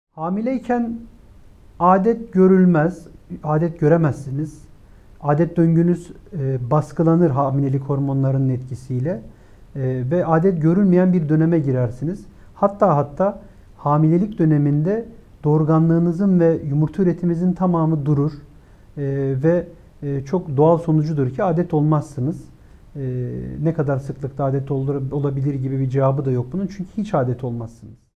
Hamileyken Adet Görülür Mü?
Cemil Karakuş sizler için anlattı:

Hamileyken adet görülmez çünkü hamilelik döngünüz baskılanır...

Bu dönemde hamileliğiniz baskılanır, hatta bu dönemde aslında tüm doğurganlık kısıtlanır.

Kısacası hamilelik döneminde regli olmazsınız...